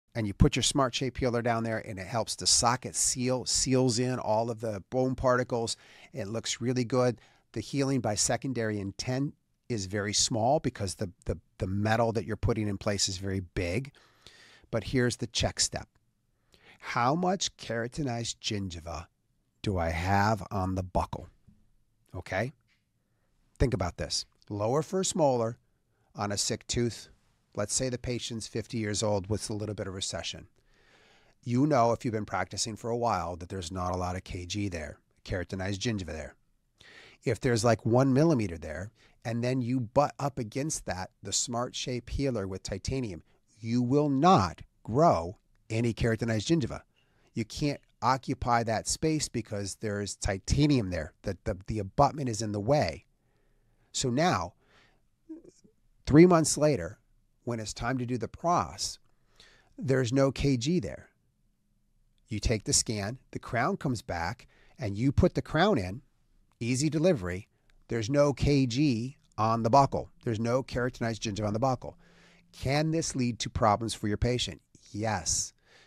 The costly mistake dentists make with SmartShape Healers — skipping the keratinized gingiva check
BioHorizons just released their new Smart Shape healers — but are they worth the hype? In this week’s episode, Dr. Robert Stanley, The Smile Engineer, breaks down the design, benefits, and potential drawbacks of these new healing abutments. He also dives deep into one of the most overlooked parts of implant success: building keratinized gingiva on the buccal to support both healing and long-term restorative outcomes.

Dr. Stanley explains why buccal keratinized tissue is so important, how to develop it effectively, and how it ties into overall implant stability and esthetics. Whether you’re already using Smart Shape healers or just curious about their impact, this video will help you make more informed decisions for your patients and your practice.